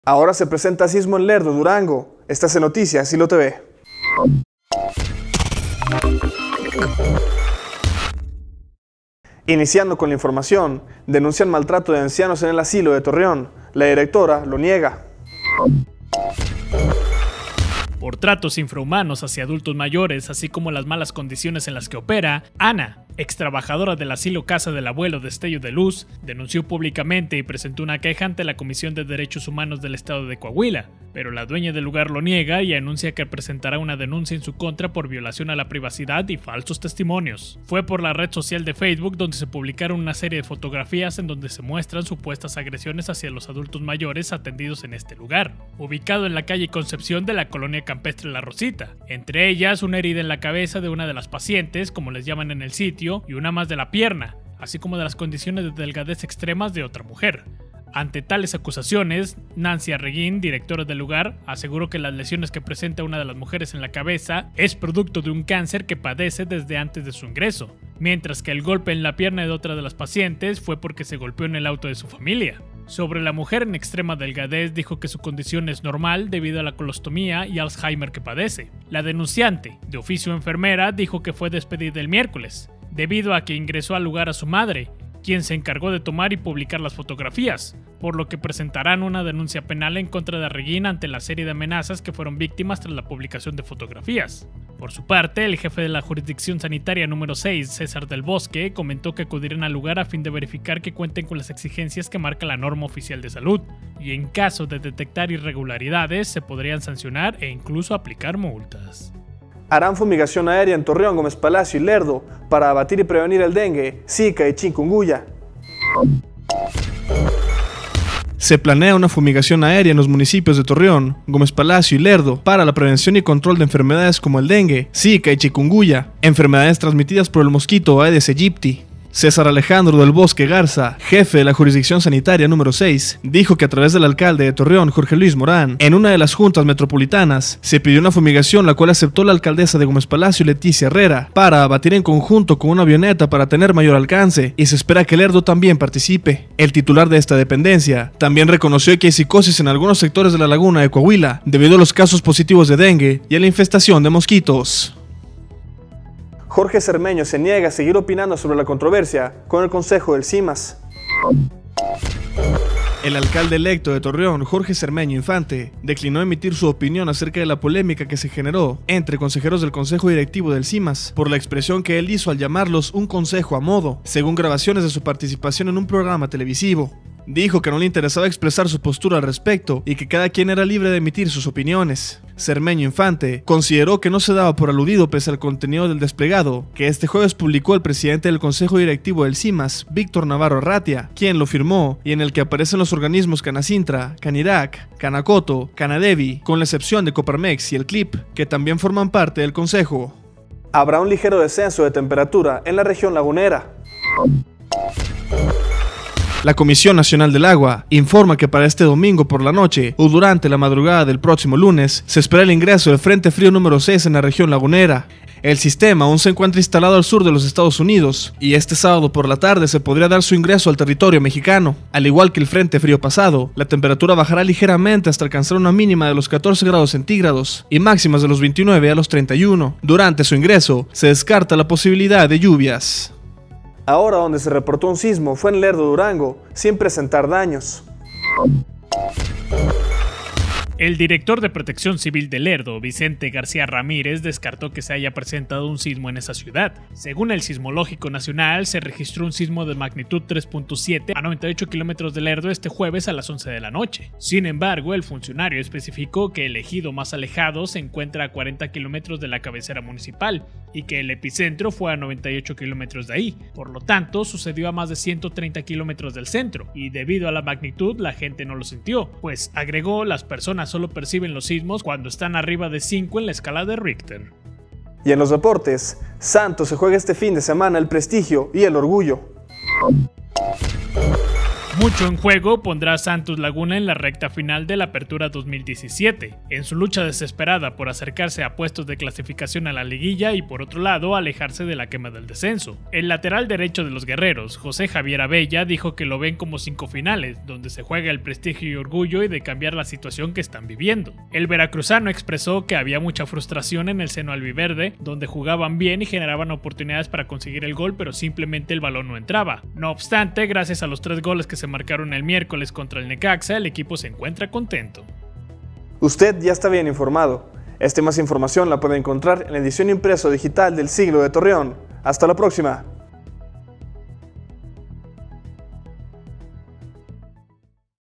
Ahora se presenta un sismo en Lerdo
::: Denuncian maltrato de ancianos en asilo de Torreón; su directora lo niega ::: Jorge Zermeño se niega a seguir opinando sobre la controversia con el consejo del Simas ::: Habrá un ligero descenso de temperatura en La Laguna ::: Santos se juega el orgullo y prestigio :::